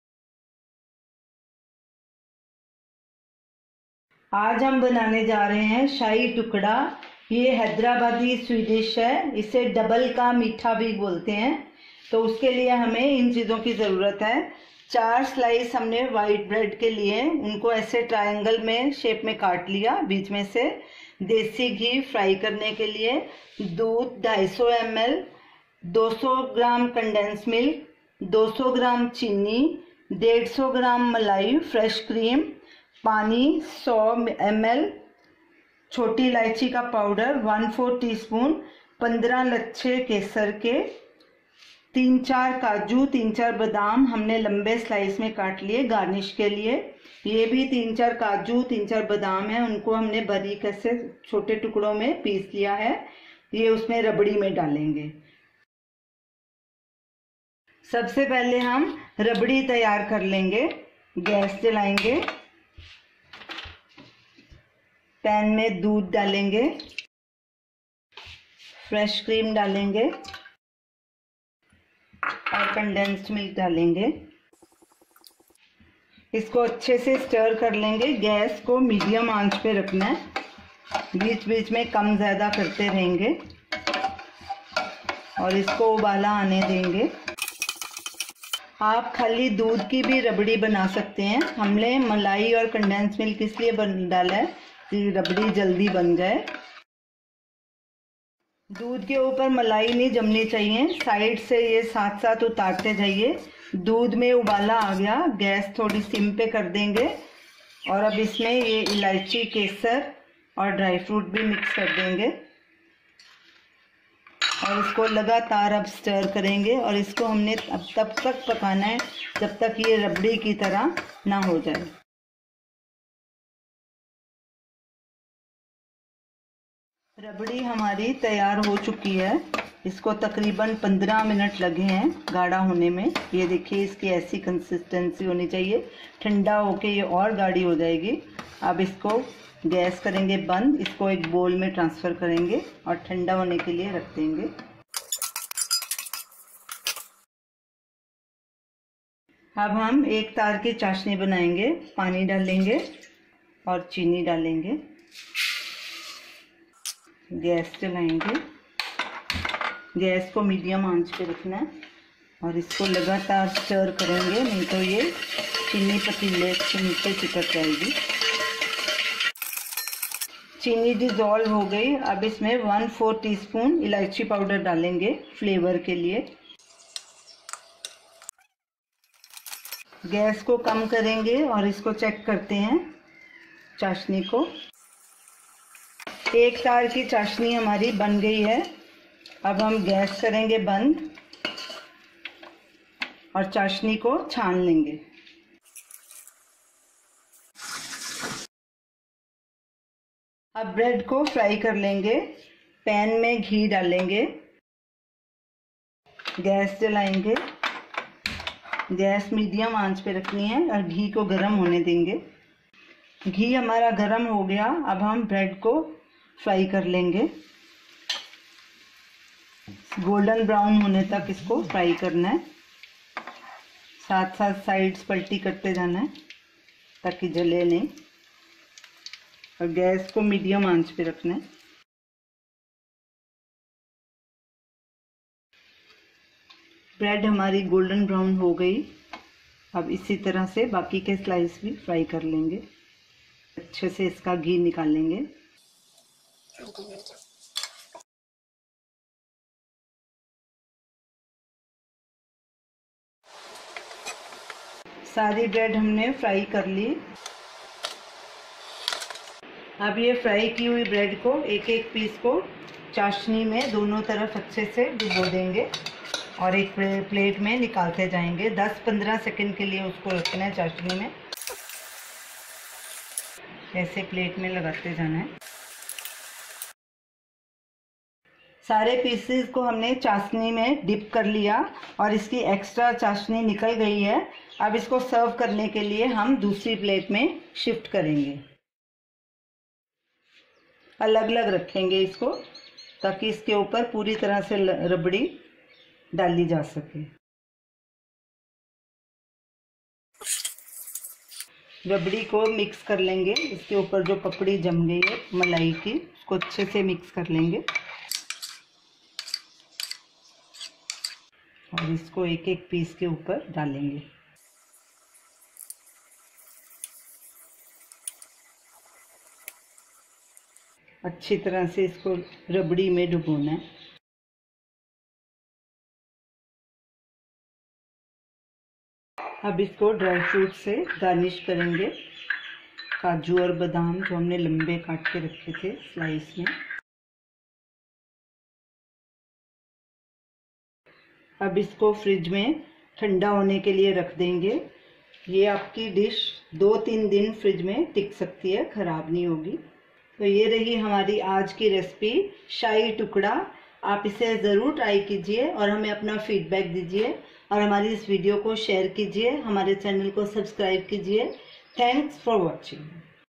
Shahi Tukda Recipe | How To Make Shahi Tukda | Double Ka Meetha Recipe | Sweet Bread Pudding
Prepare Shahi Tukda, the royal Hyderabadi sweet dish, with this step by step easy recipe. A Mughlai dessert from the royal cuisine. Do try it and share your feedback.

Ingredients
for the rabri
250 ml full fat milk
150 grams malai/cream
200 grams condensed milk.we have used homemade
12-15 kesar/saffron strands
1/4 tsp cardamom powder
4-5 cashews, chopped small
4-5 almonds, chopped small

for the sugar syrup
200 grams sugar
1/2 cup/100 ml water
1/4 tsp cardamom powder

for frying the bread
desi ghee/clarified butter as needed
4 white bread slices. you can use brown bread instead

Method
Prepare the rabri first
1. to a pot add the milk, malai & condensed milk. on medium heat keep stirring & cooking till the milk comes to a boil & the ingredients mix together
2. add the cardamom powder, the chopped dry fruits & the saffron
3. cook till the mixture becomes thick like rabri as shown in the video
4. transfer to a bowl and allow to cool down

Prepare the sugar syrup
1. to a pot add the water & sugar
2. on medium heat stir constantly till sugar dissolves
3. add the cardamom powder & cook till the syrup gets to a 1-string consistency
4. remove to a bowl & keep aside

fry the bread slices
1. cut each bread slice diagonally across or as you wish
2. add enough desi ghee to a pan & heat on medium heat
3. add 1 or 2 bread slices and fry them till crisp & golden. keep turning them frequently to fry evenly on both sides
4. transfer to a plate when done & fry all the bread slices likewise

assemble the shahi tukda
1. dip the bread slices in the sugar syrup one by one for about 10-15 seconds each turning them over as well
2. hold them against the bowl to let the syrup drain out & keep transferring them to a plate.
3. when you are done with the dipping you will find more syrup has drained from the slices out in the plate
4. arrange the slices in another dish or plate and pour the now cooled rabri liberally over them
5. garnish with the sliced dry fruits & keep the shahi tukda in the fridge for a few hours to chill then serve